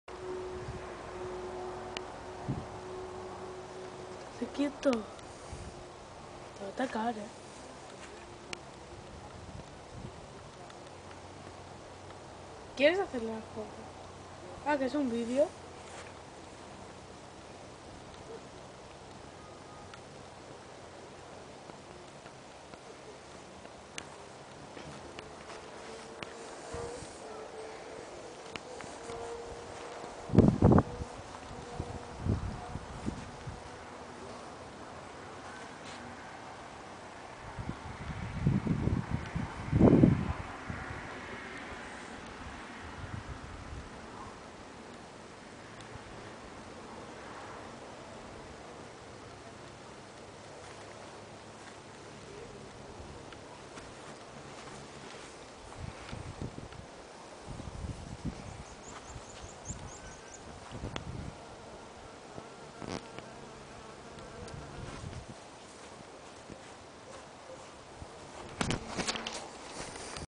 ALIEN EN LAS HABAS?????????
ALGUIEN ME PUEDE DECIR QUE INSECTO ES ESTE?ES UN ABEJORRO? ES UN COLIBRI? ES UNA ALIEN DISFRAZADO DE PAJARO-MOSCA? O ES EL HIJO SECRETO DE RAJOY?